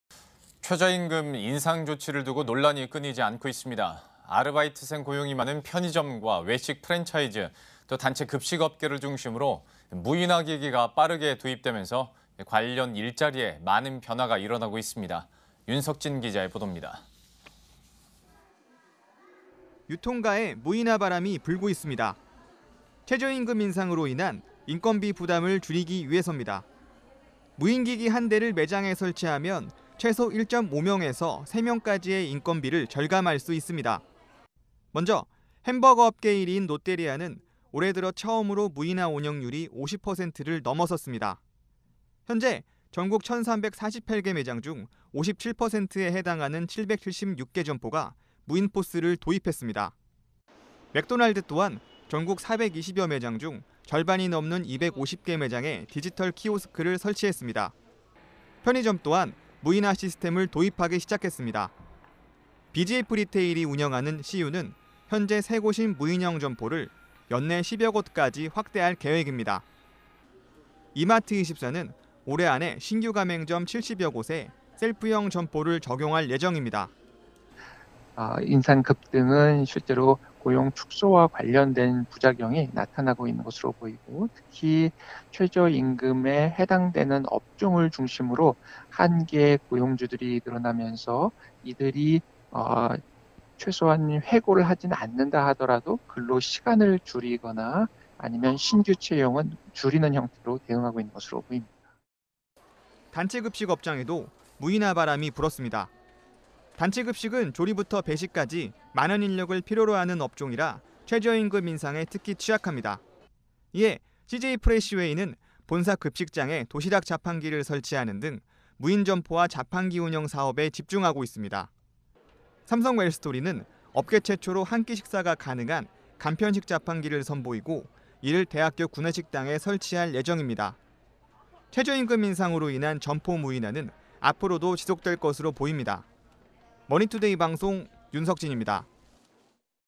유통가, 최저임금 후폭풍 직격탄…사람 밀어내는 무인 기계
07/18 이슈&뷰11

최저임금 인상 조치를 두고 논란이 끊이지 않습니다. 아르바이트생 고용이 많은 편의점과 외식 프랜차이즈, 단체 급식 업계를 중심으로 무인화 기기가 빠르게 도입되면서, 관련 일자리에 많은 변화가 일어나고 있습니다. 윤석진 기자의 보도입니다.